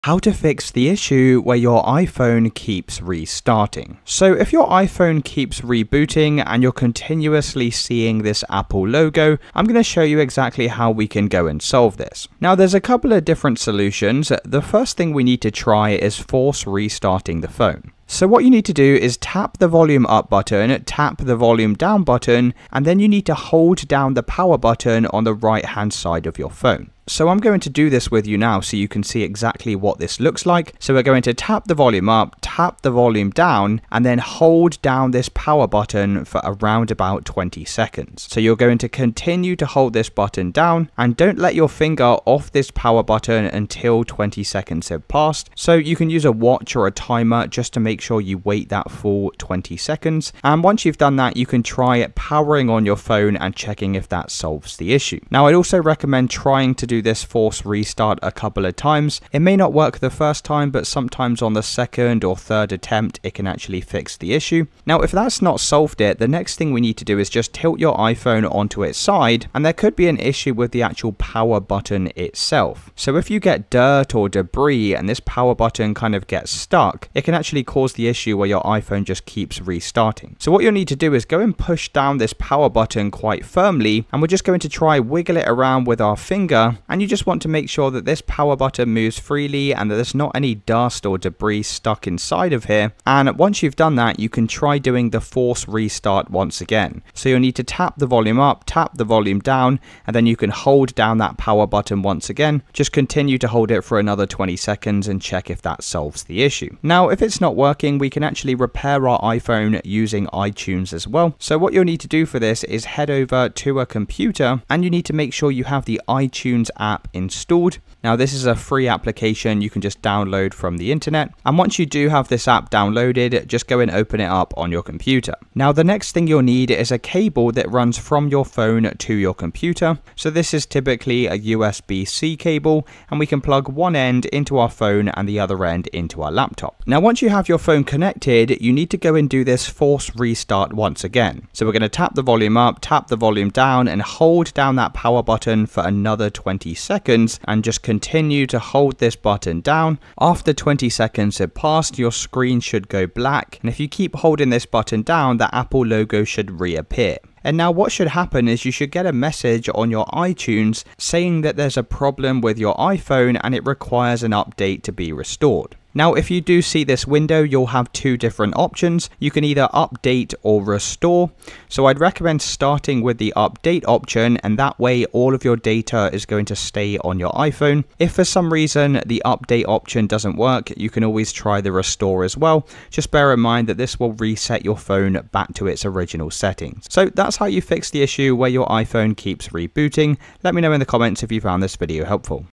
How To Fix iPhone Keeps Rebooting With Apple Logo
Fix an iPhone stuck in a reboot loop or frozen on the Apple logo. This guide covers force restarts, recovery mode, and iTunes restore options. Works for iPhone 12, 13, 14, and later. Stop your iPhone from restarting constantly and get it working again.